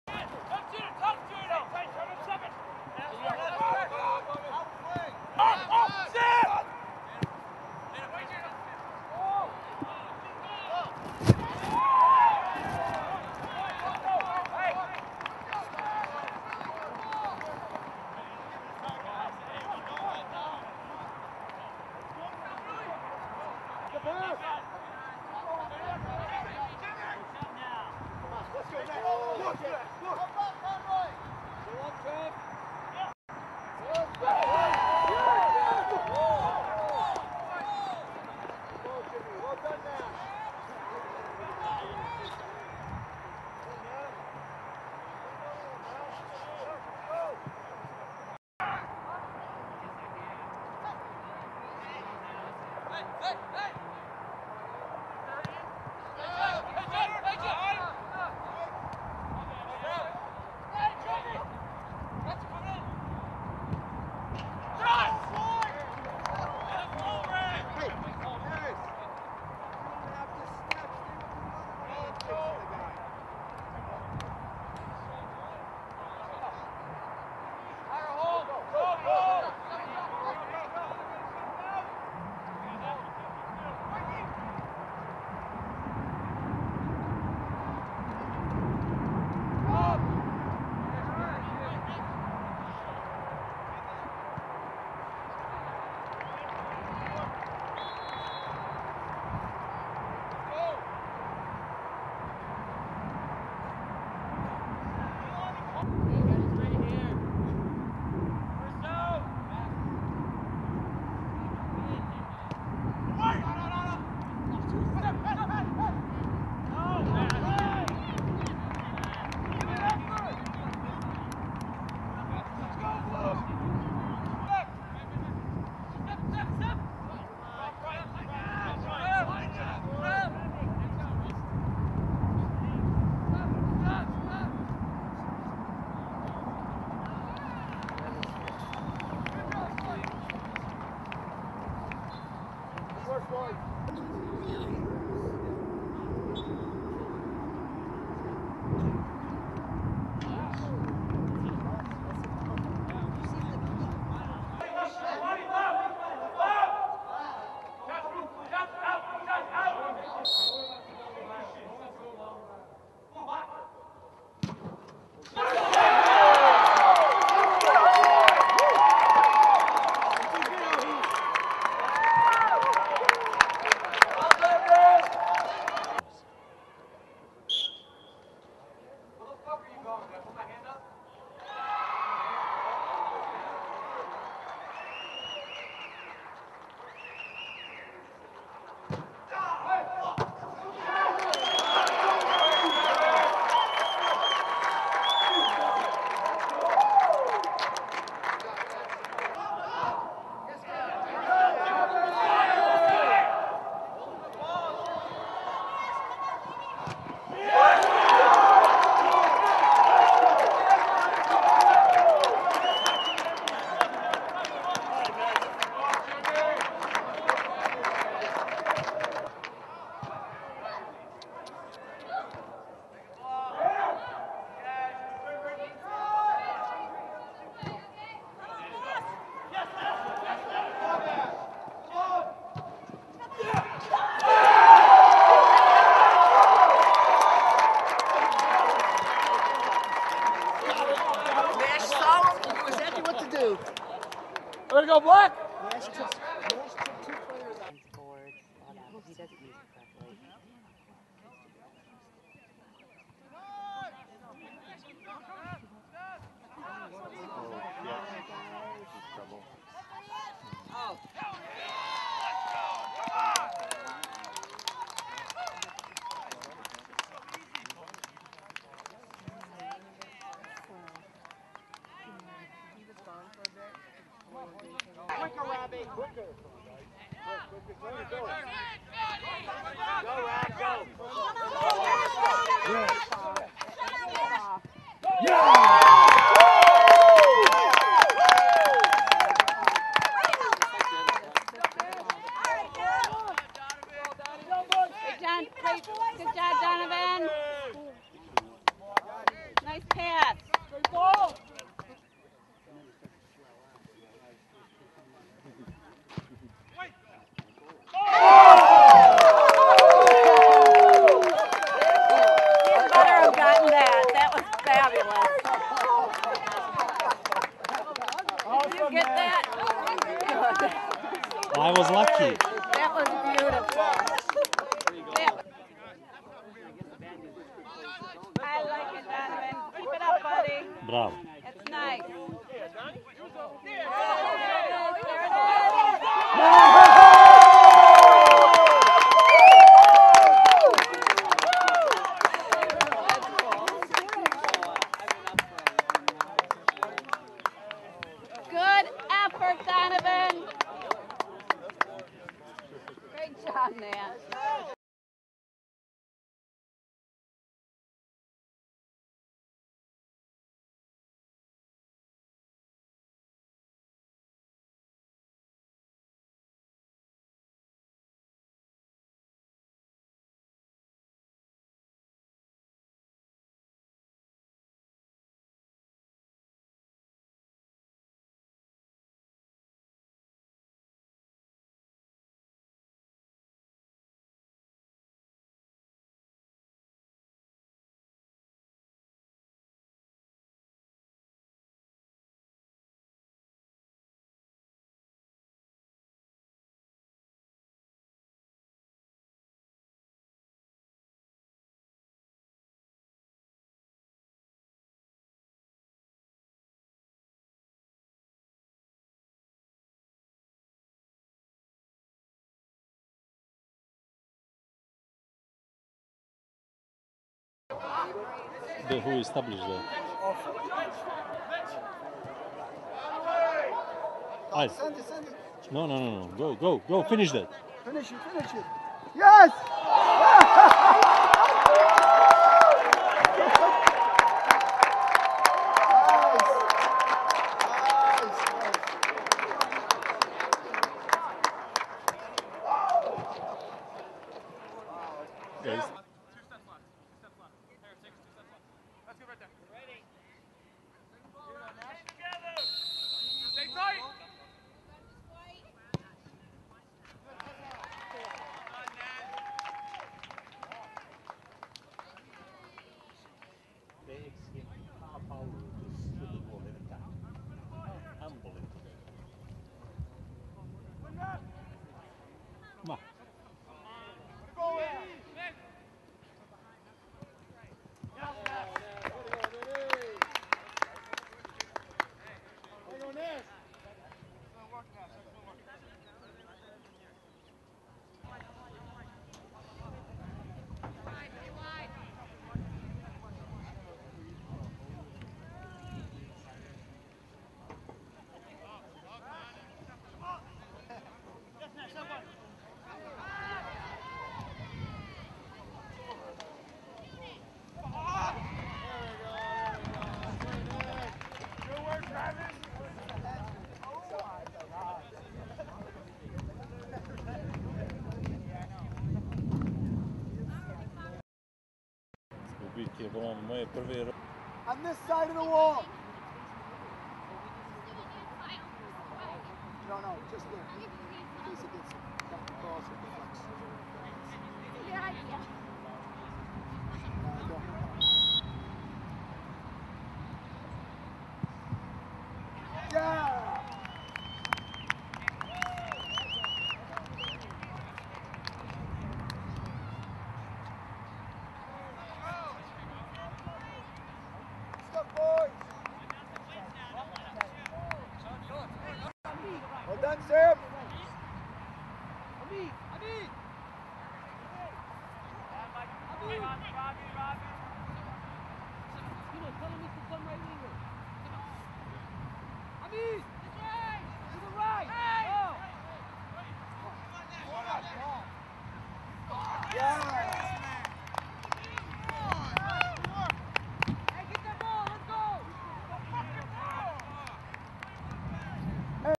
2014-15 Highlights Dad Filmed
Nebojsa Popovic's Highlights from 2014-15 Academy Season playing for
Vardar U18s.